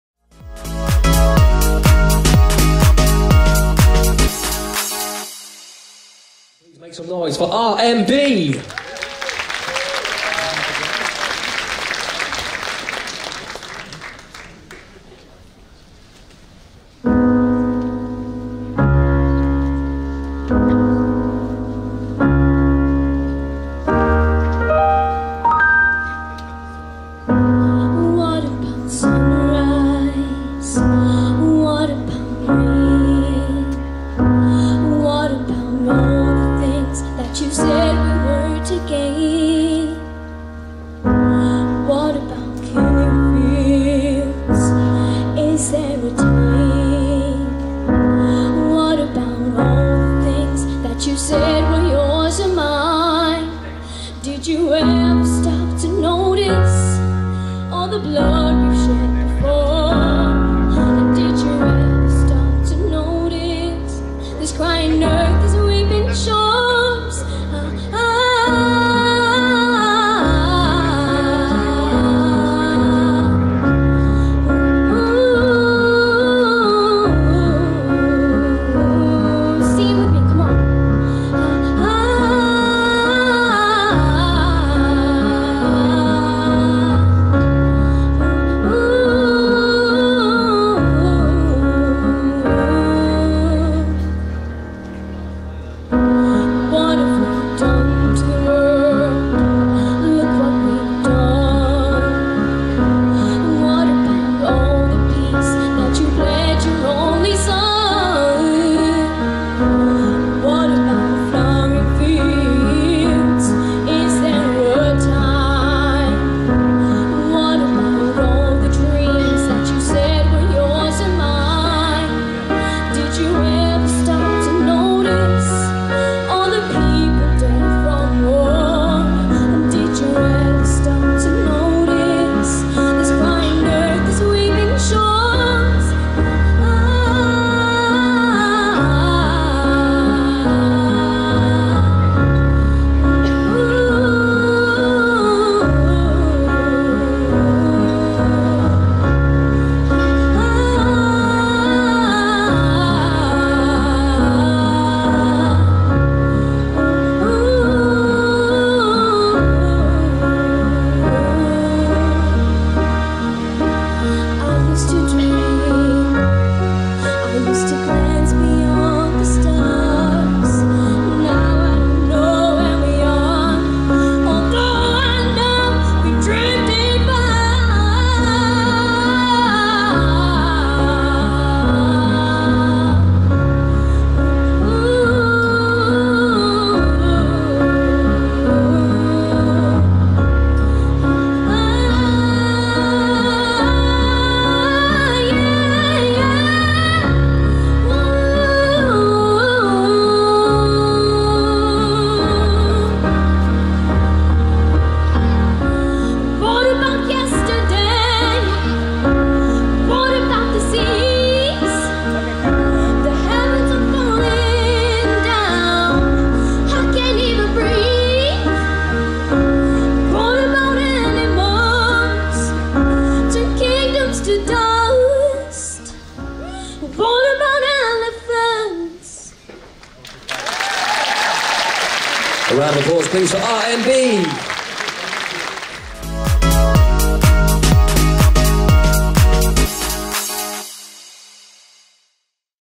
AREEMBEE (RMB) at Grand Final of TeenStar Singing & Dance competition
TEENSTAR AREMEBEE (RMB) performance at the TeenStar Singing & Dance Competition Grand Final.